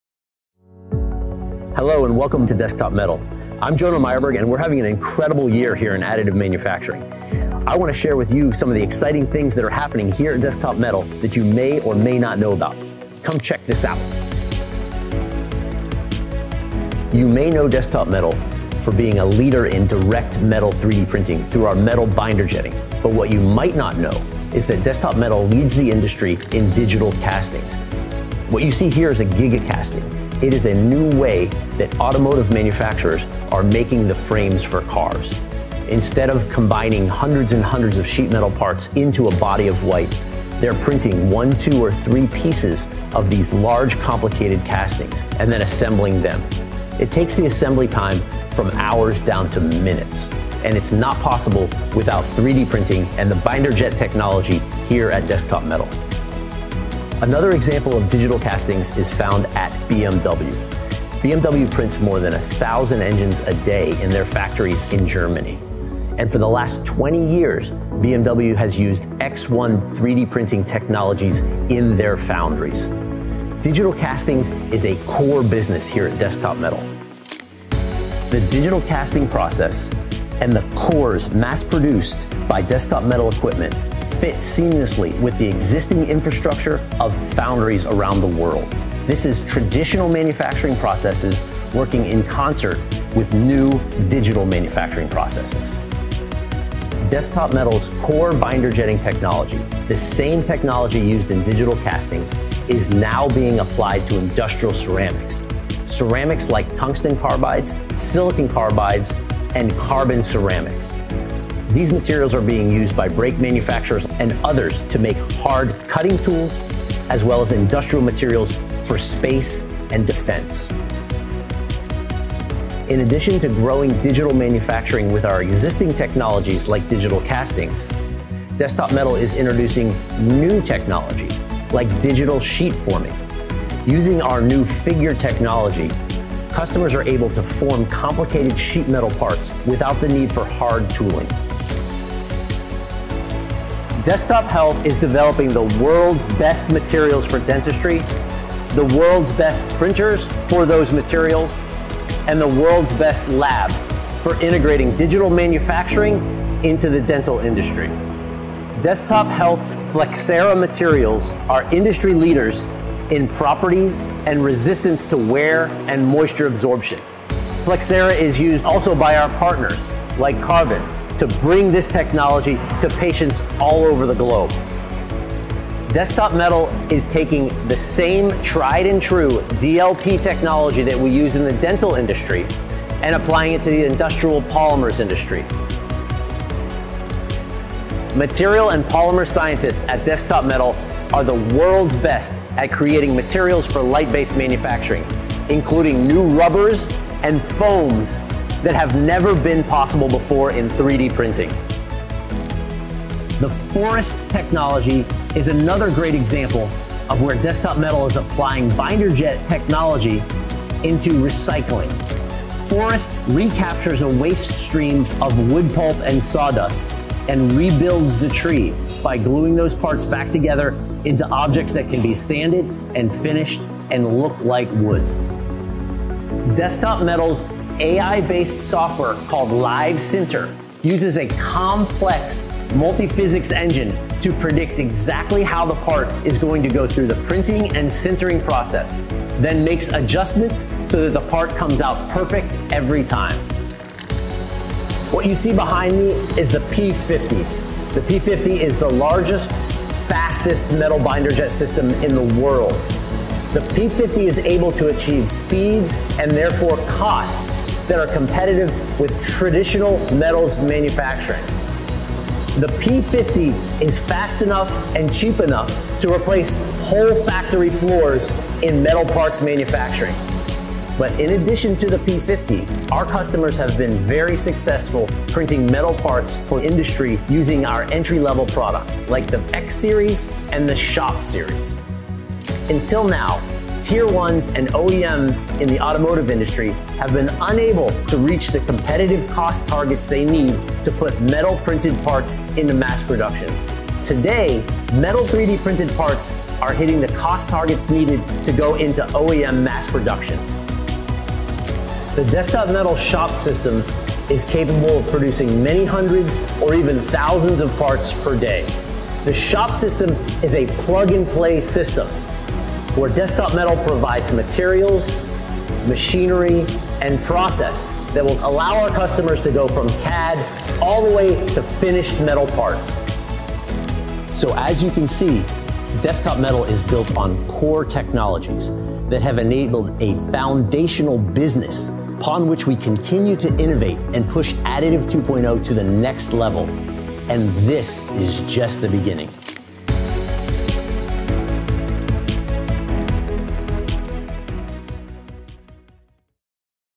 Pushing Additive 2.0 to the Next Level
TeamDM is passionate about driving Additive Manufacturing into its next era of mass production. The good news: we're well on our way! Take a look inside our AM 2.0 universe making metal, ceramic, and polymer end-use parts in real production environments. Manufacturers are now scaling AM from prototypes and batch volumes to mass production.